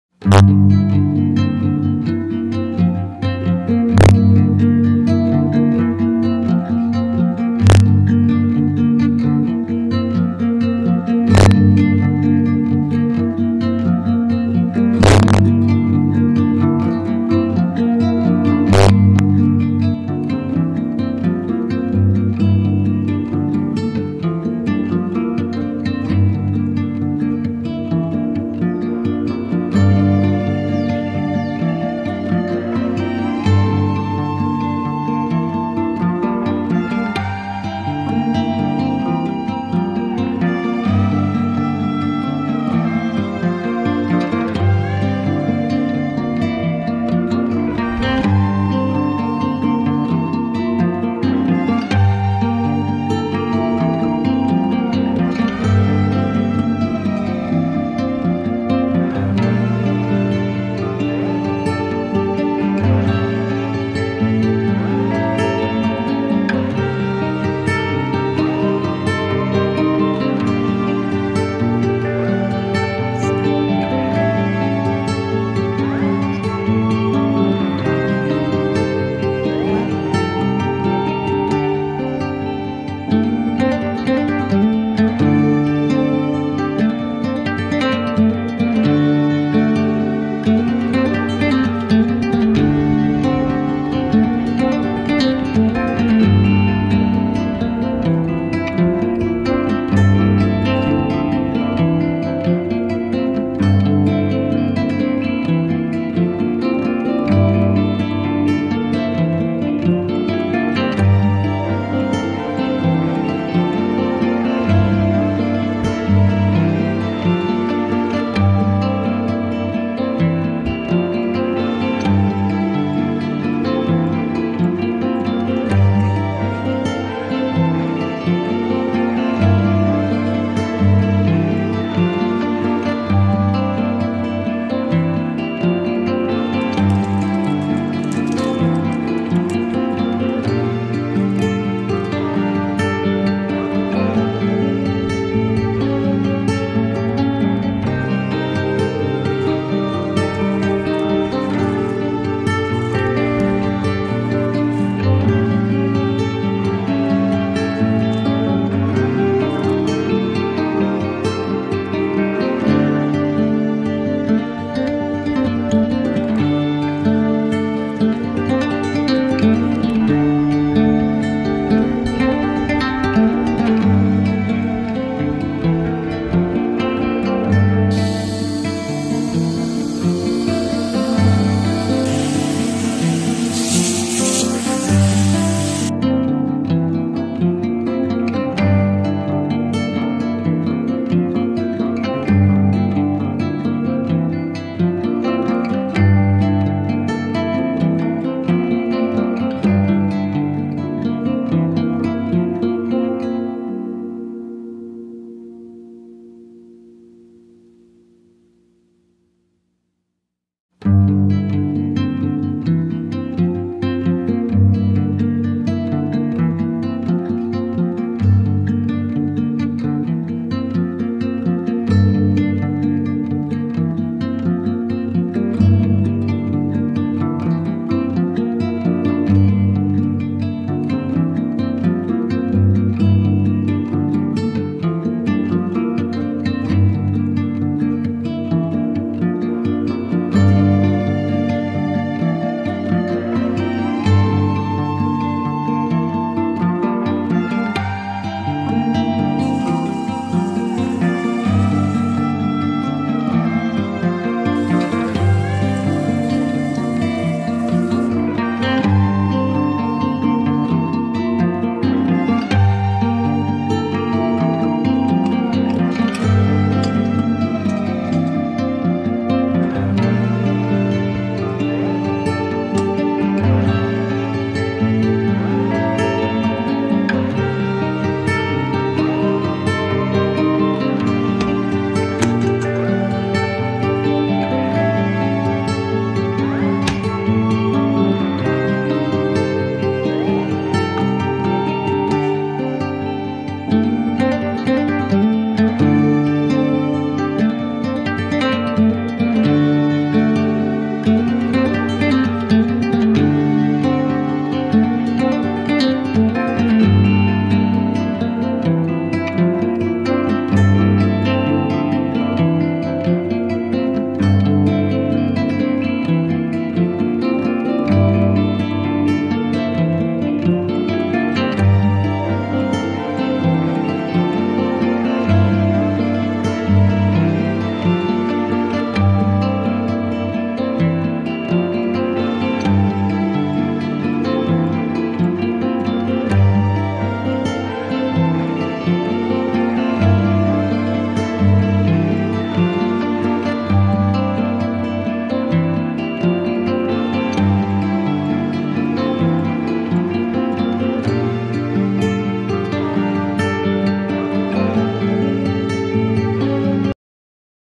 This Suburban...Peaceful
This is my daily life in suburban area in North California